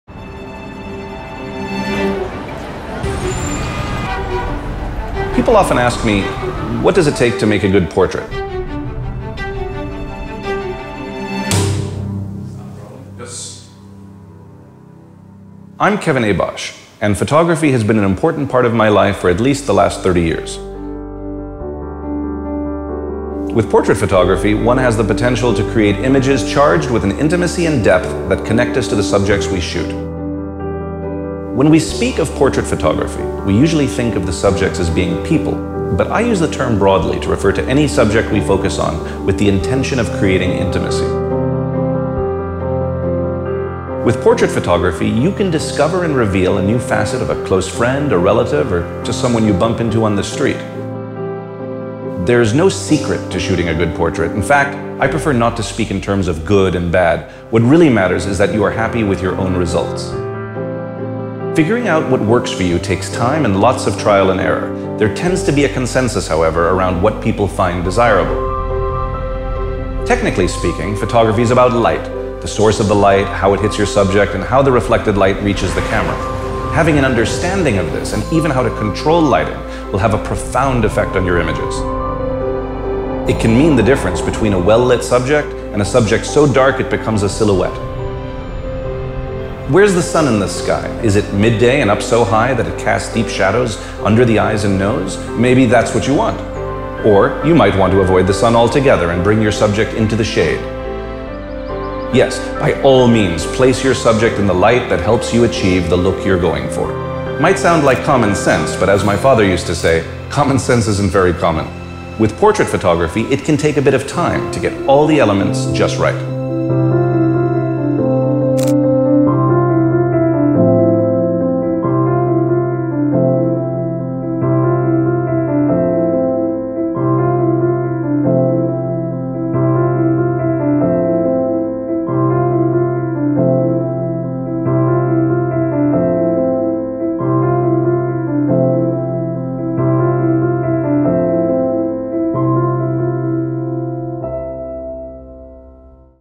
OnePlus 5 – Discovering portrait photography with Kevin Abosch
Kevin Abosch aims to do for portraits what we do for smartphones, crossing the gap between good and great. What makes a portrait special? How do you frame a person? Find answers to these and many other questions in this journey into the art of Kevin Abosch and the power of Portrait Mode on the OnePlus 5. Ready to shoot your own portraits?